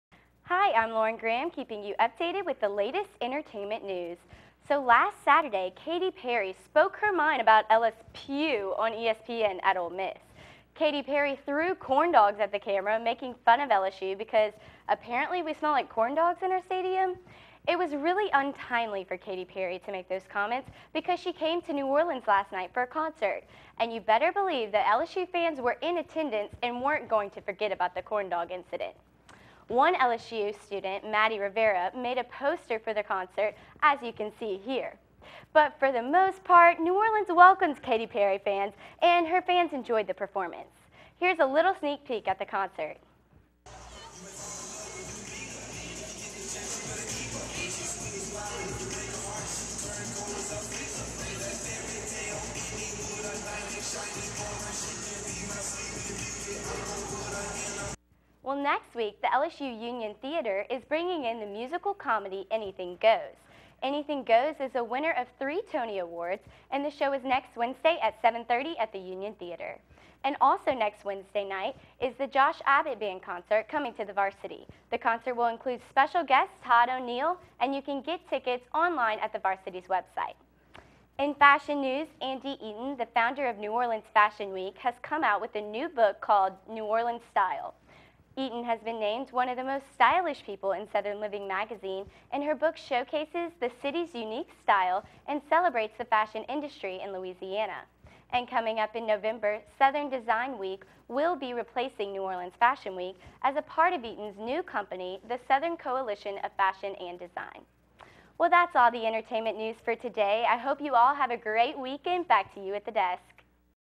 Entertainment Report 10/9/14 - Newsbeat
Lauren Graham updates on what's new in entertainment news.